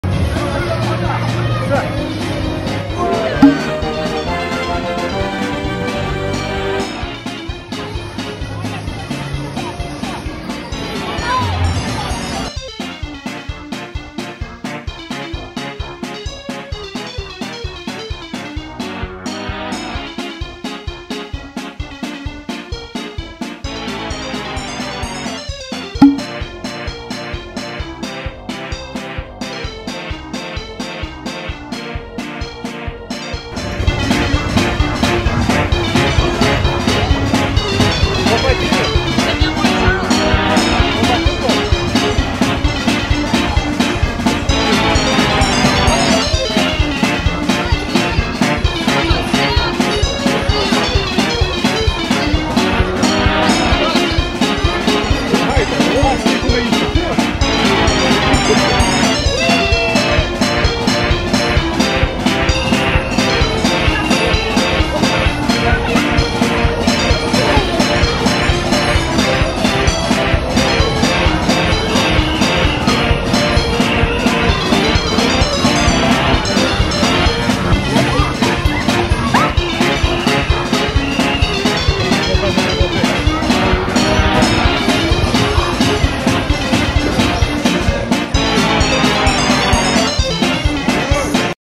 Renea plays at Indoor Playground Family Fun Play Area for Kids fun Play time
Jumping and playing